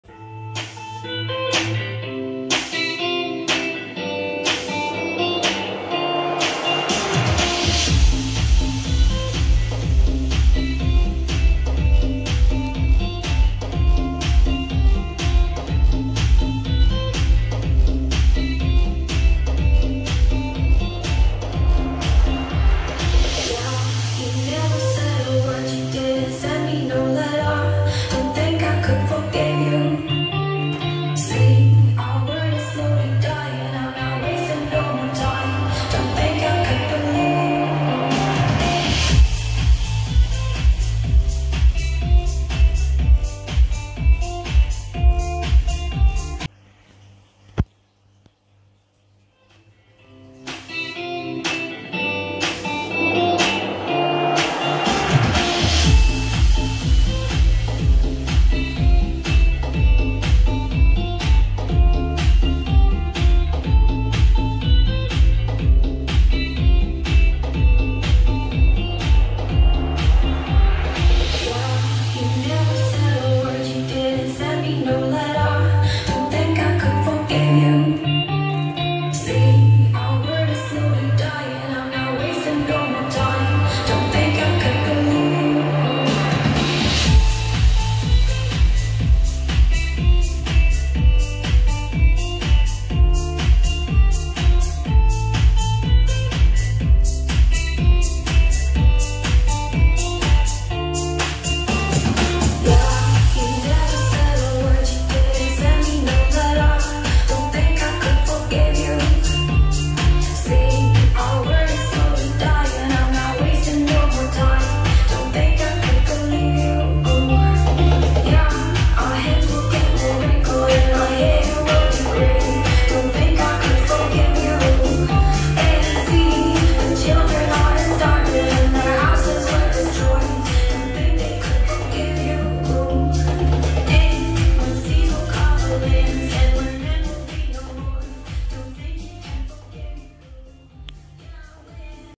Jbl partybox 310 vs partybox 310 tws
Hi everyone i just thought i make a vid of my partybox 310's, first in mono or single then in tws.

Sorry for poor sound quality my phones mic is not very good🙈